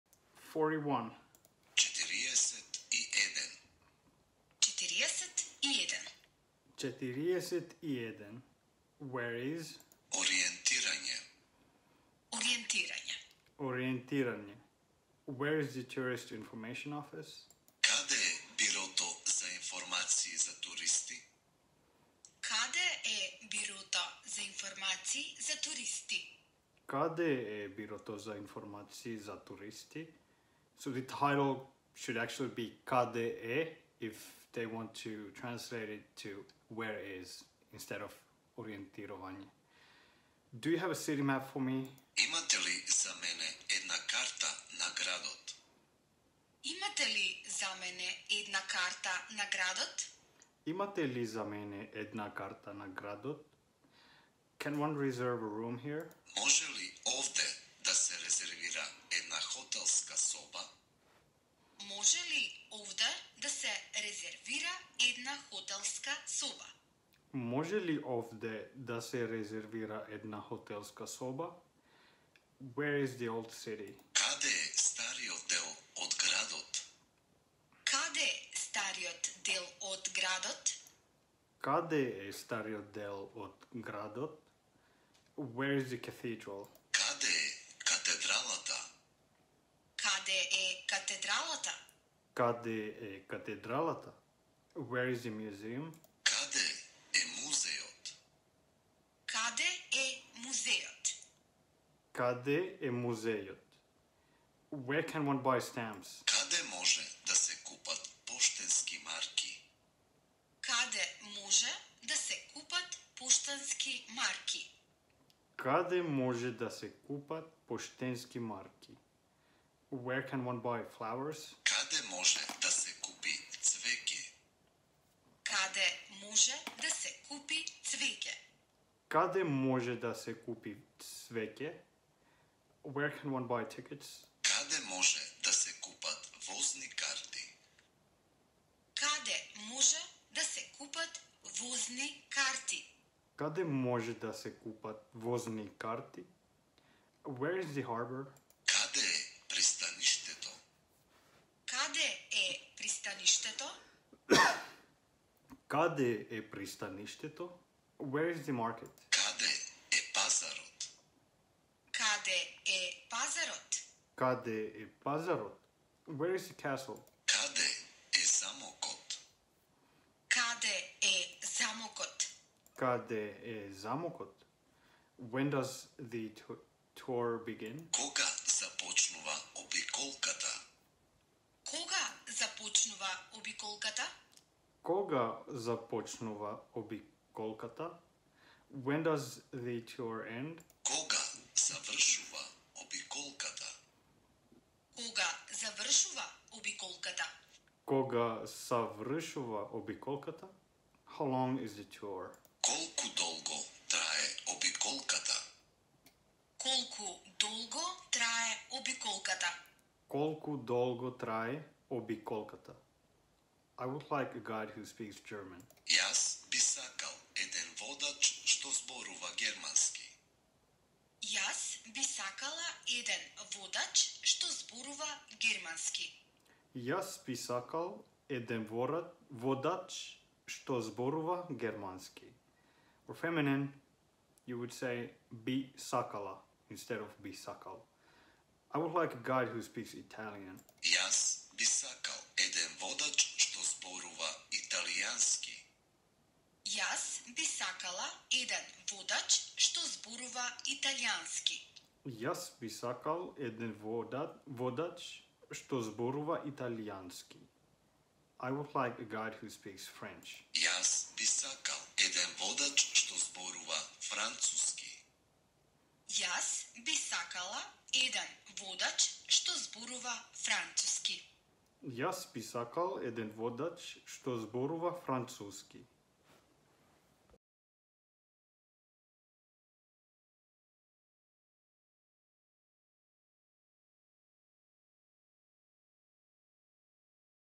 Learning Macedonian #41 Where is ..?
I'm not a native Macedonian speaker, I'm just sharing my Macedonian learning sessions with you using this I recorded these lessons while my one month stay in Ohrid, North Macedonia in November 2021.